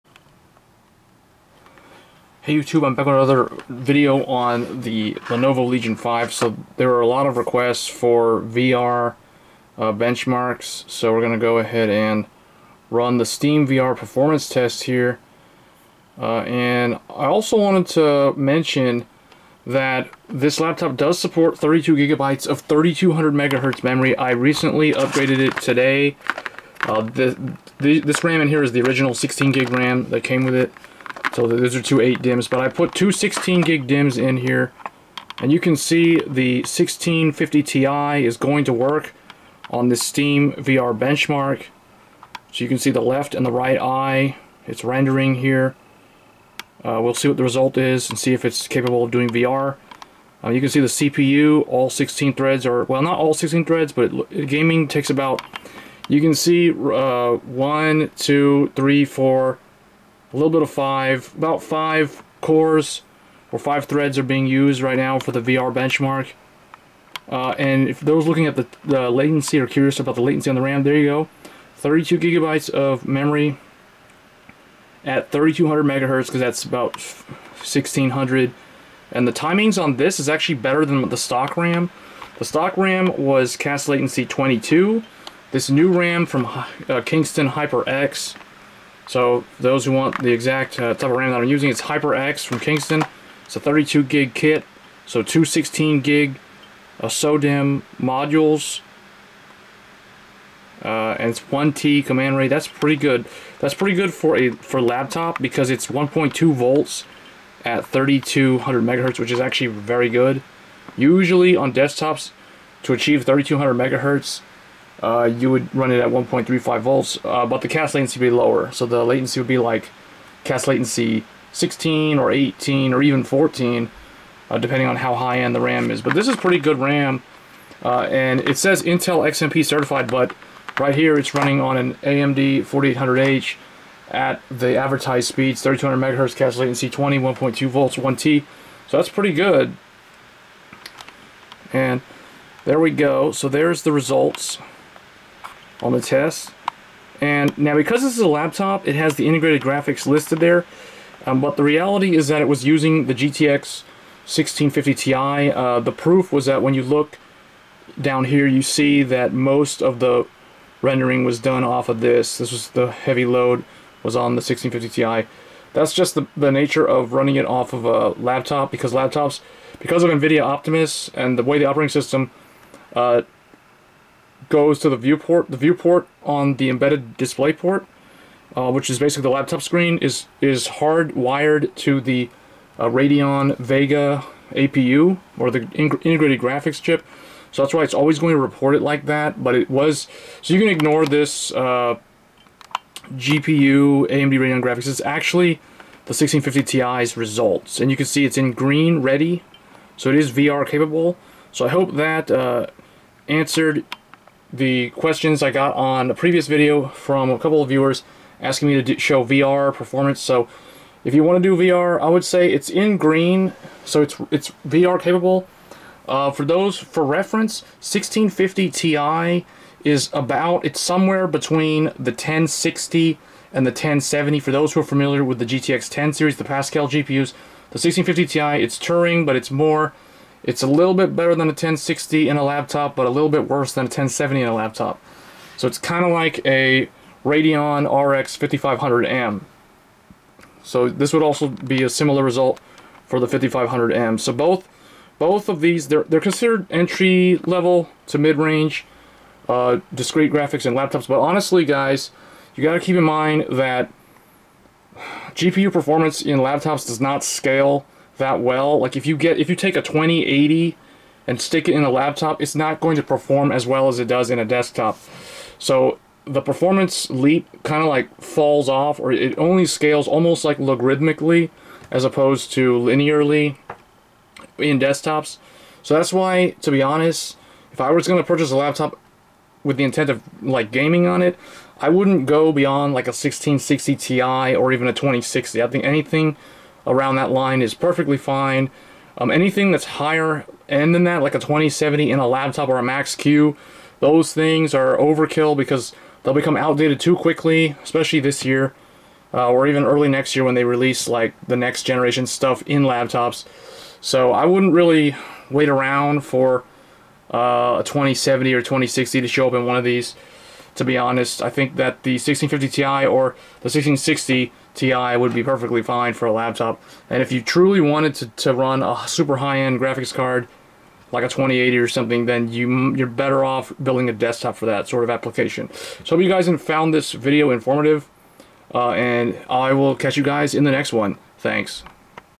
Lenovo Legion 5 AMD Steam VR Benchmark test and Laptop Memory Upgrade
Can the Legion 5 with Ryzen 7 4800H handle VR gaming? Let's find out.